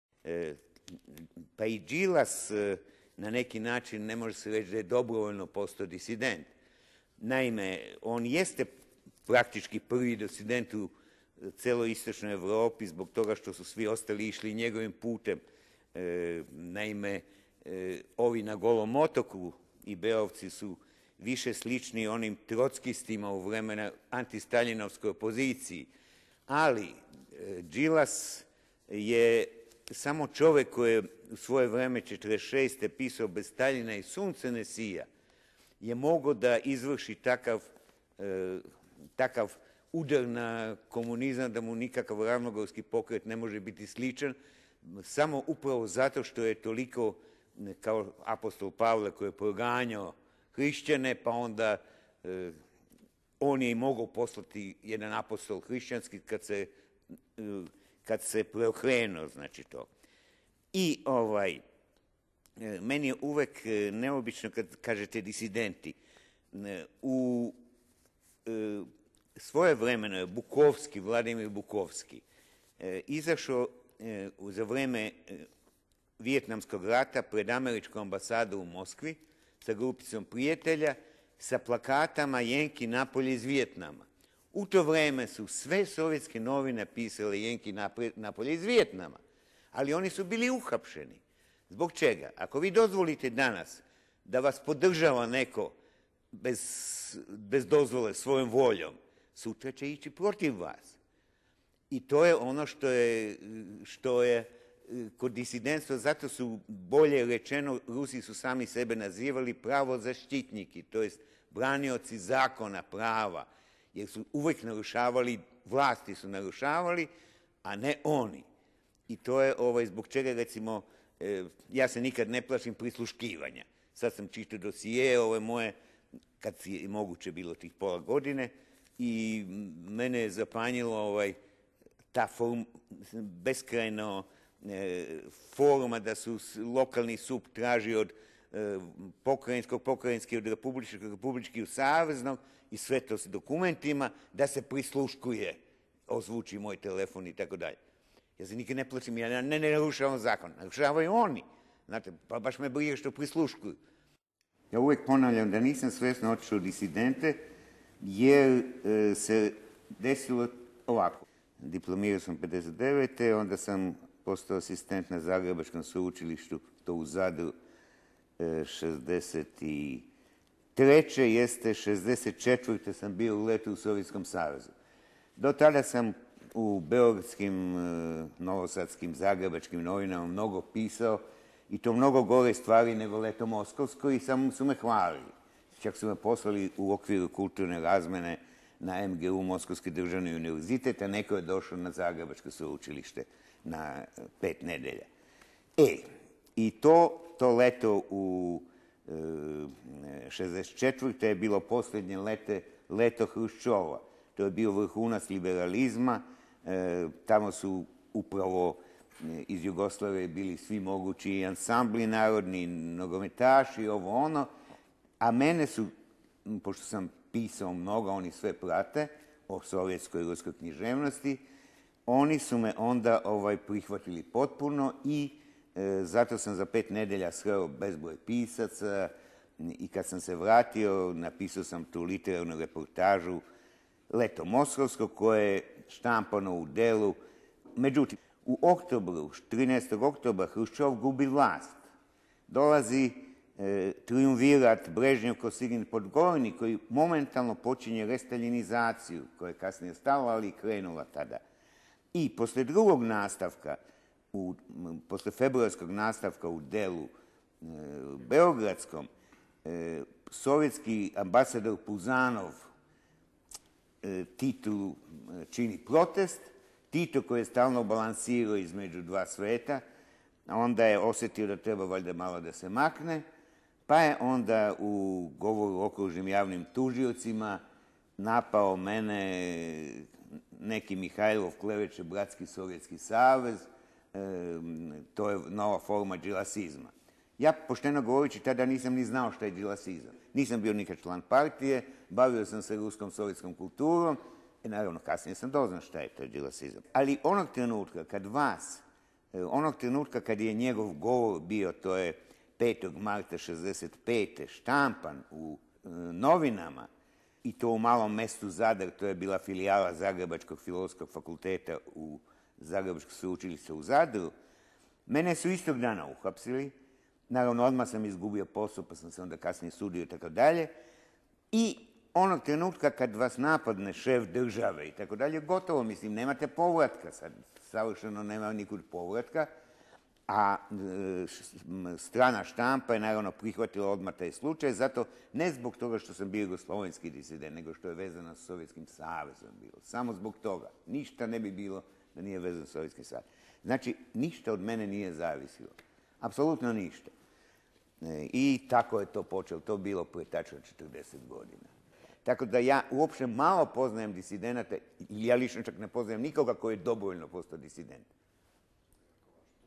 "Slobodna kultura", Mihajlo Mihajlov
Mihajlo Mihajlov, pisac, govori o disidentstvu u SFRJ